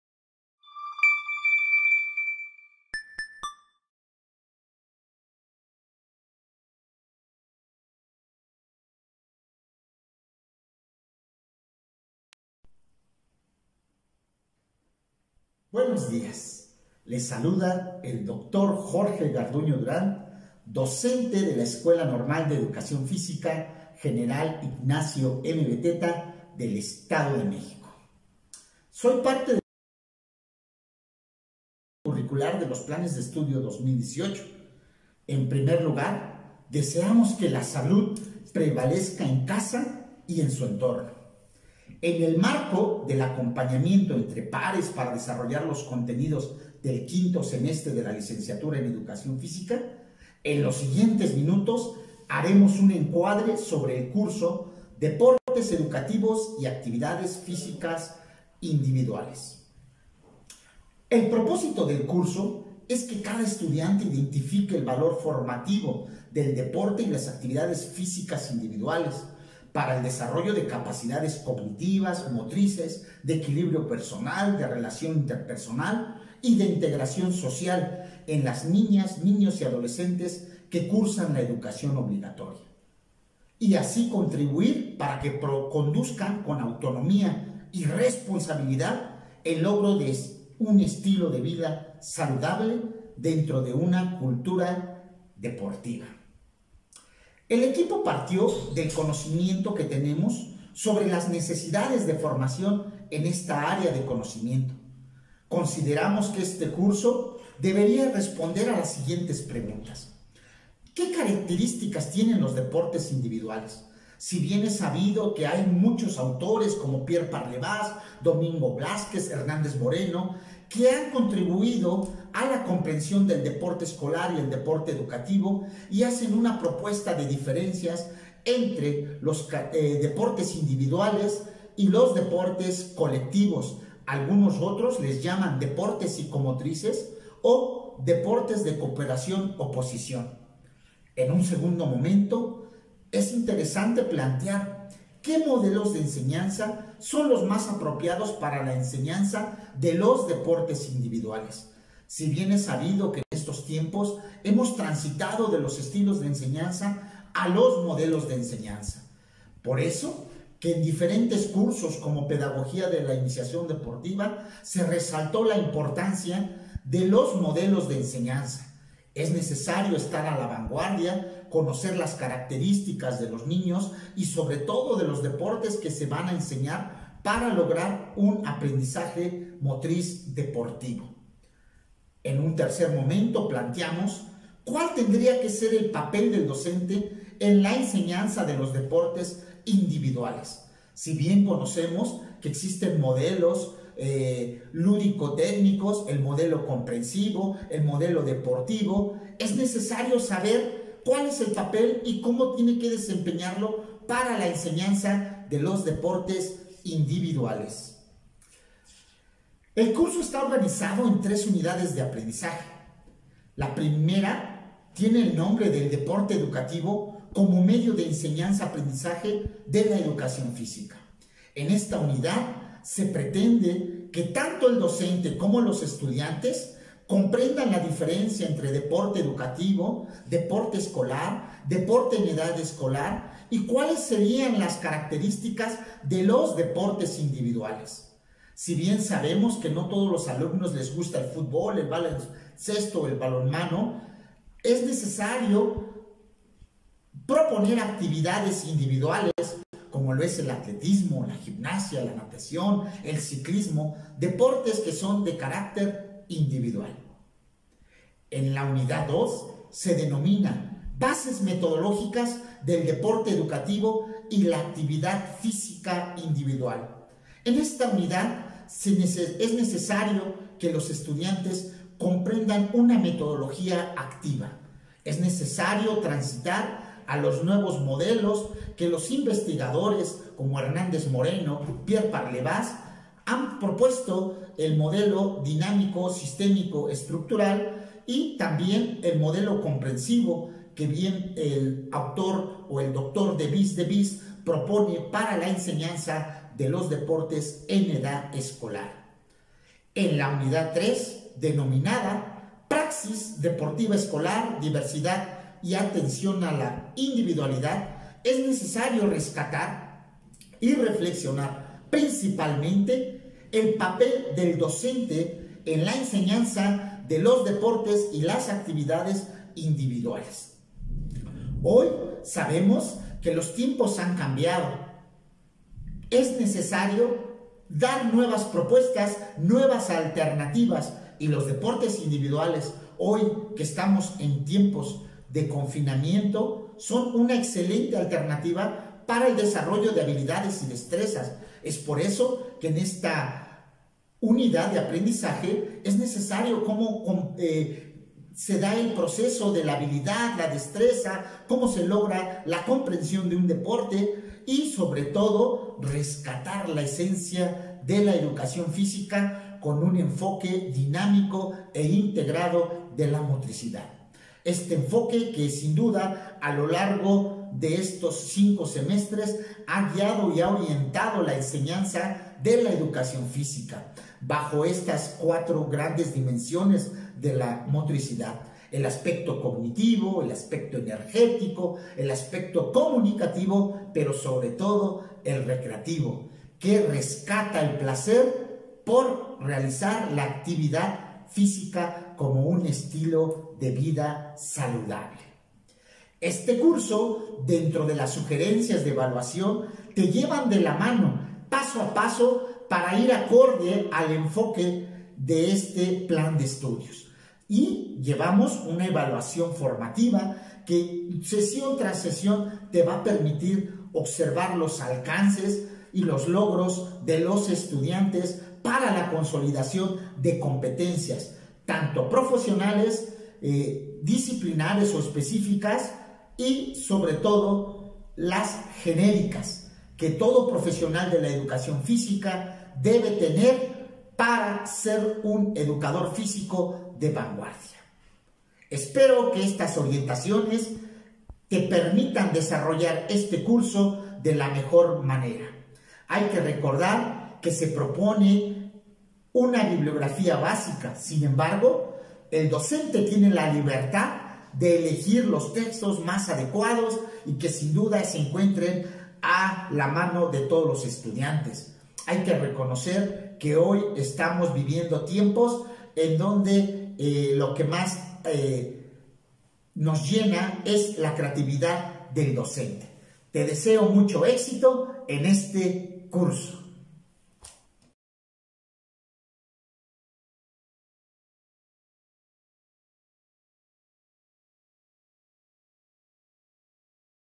Deportes Educativos y Actividades Físicas Individuales
LICENCIATURA EN EDUCACIÓN FÍSICA

5° SEMESTRE

Trayecto formativo:
Formación para la enseñanza y el aprendizaje

Este curso tiene como propósito que cada estudiante identifique el valor formativo del deporte y las actividades físicas individuales para el desarrollo de capacidades cognitivas, motrices, de equilibrio personal, de relación interpersonal y de integración social en las niñas, niños y adolescentes que cursan la educación obligatoria y así contribuir para que se conduzcan con autonomía y responsabilidad el logro de un estilo de vida saludable dentro de una cultura deportiva.